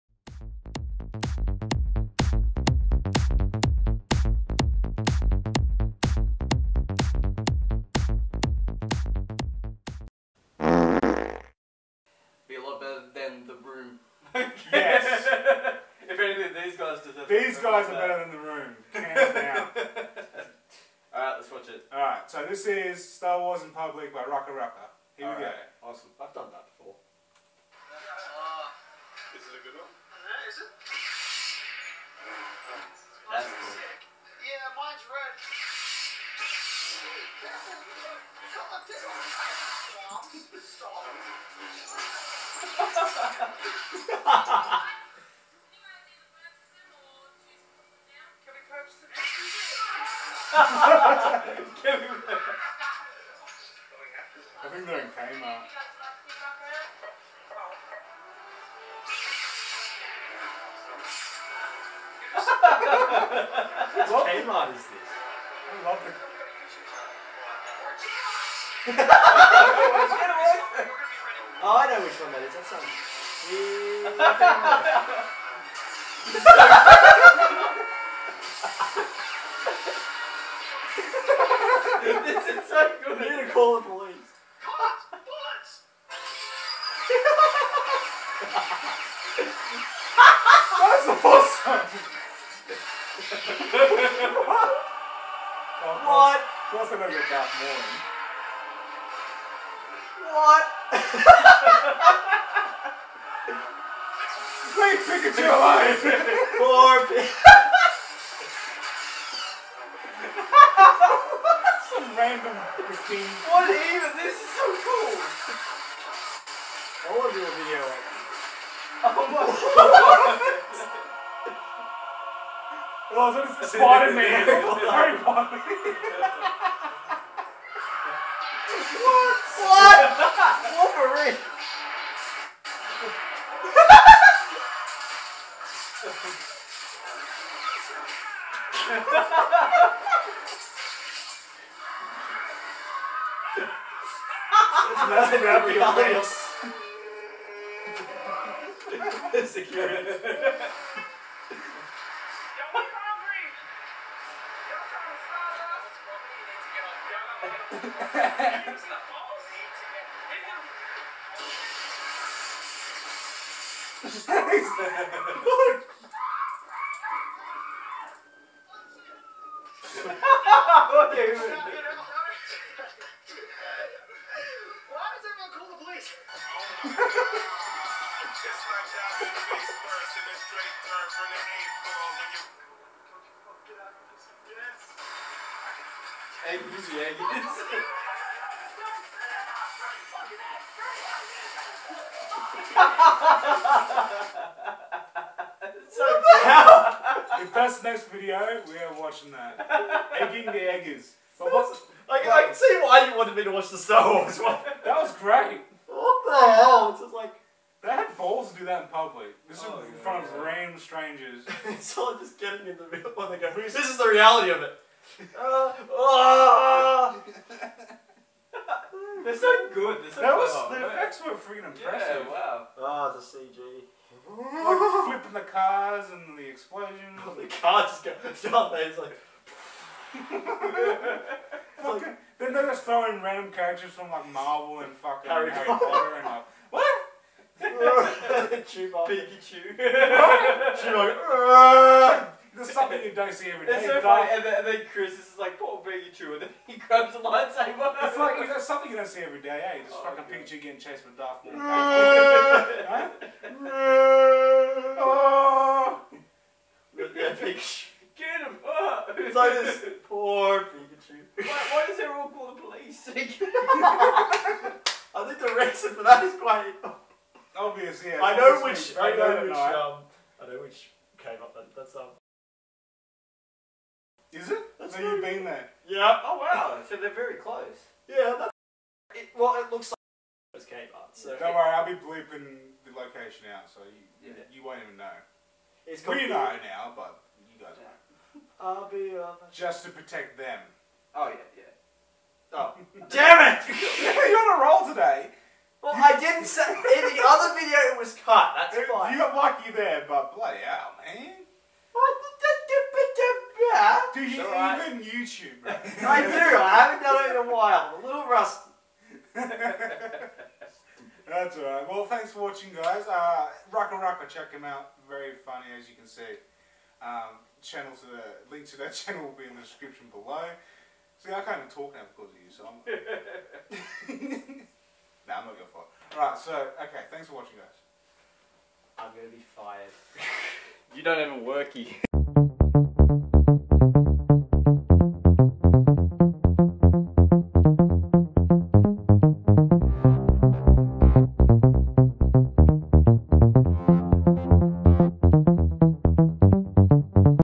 Star Wars In Public - REACTION! by Three Random Auzzies.
RackaRacka take to the streets as Jedi, Sith, Marvel Characters... Pikachu... for some reason, anyway, and go completely crazy with Lightsabers and shit!